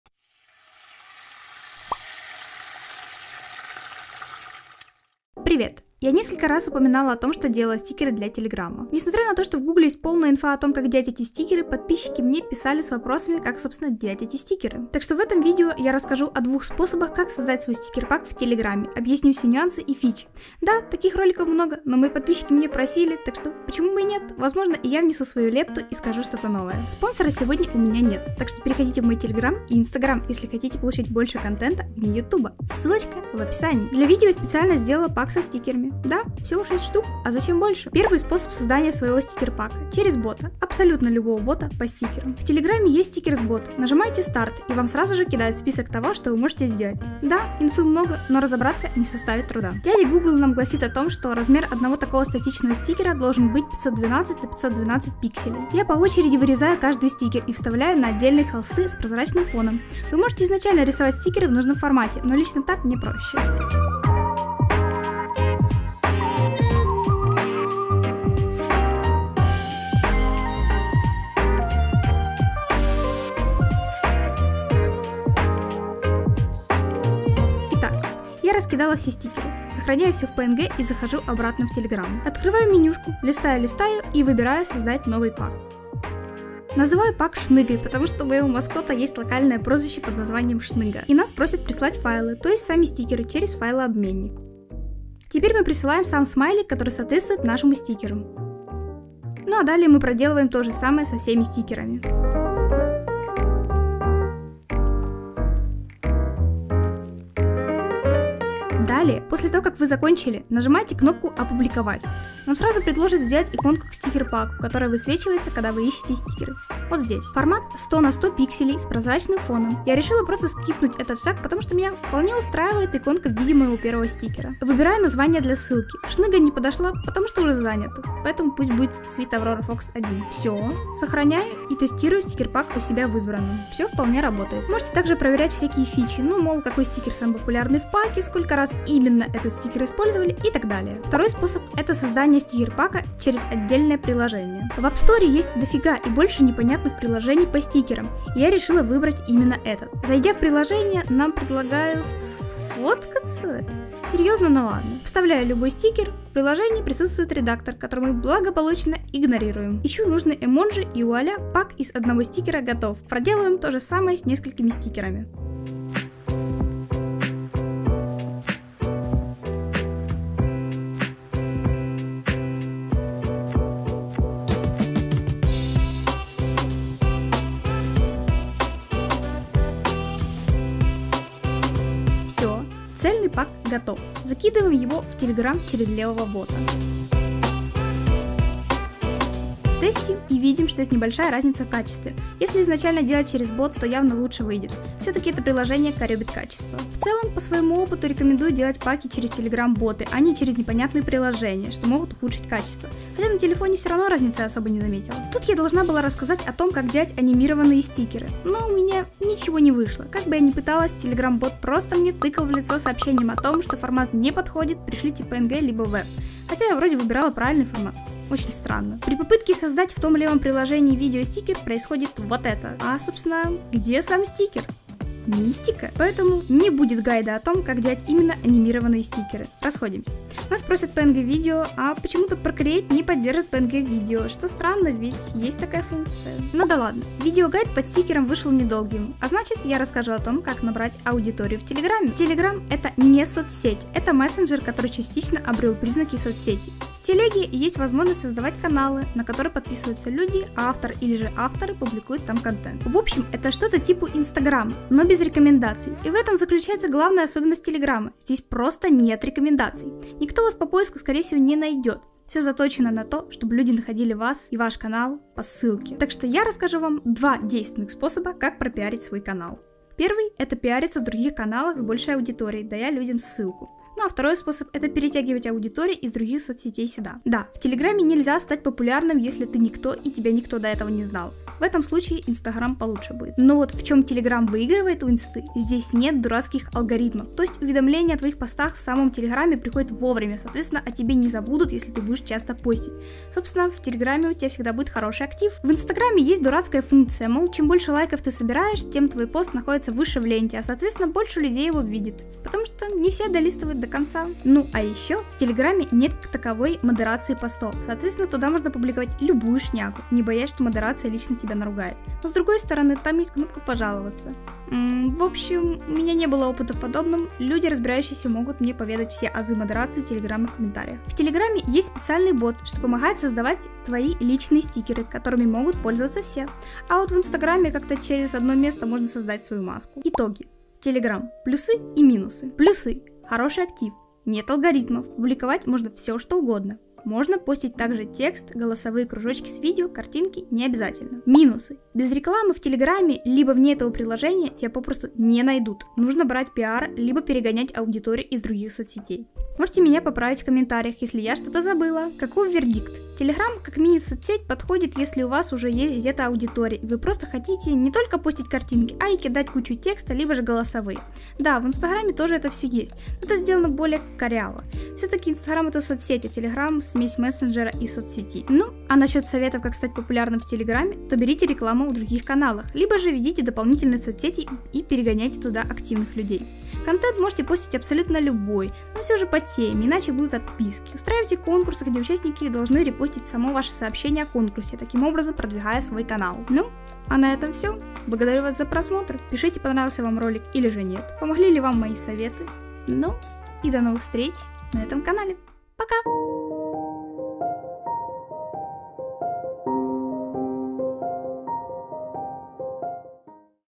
Стикеры для телеграмма | как делать + советы по продвижению | TELEGRAM
Стикеры для телеграмма | как делать + советы по продвижению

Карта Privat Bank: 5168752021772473

Эпизоды
0:00 Вступление
0:38 Через бота по стикерам
2:21 Через приложение
3:56 Как набрать аудиторию в Telegram
5:30 Итоги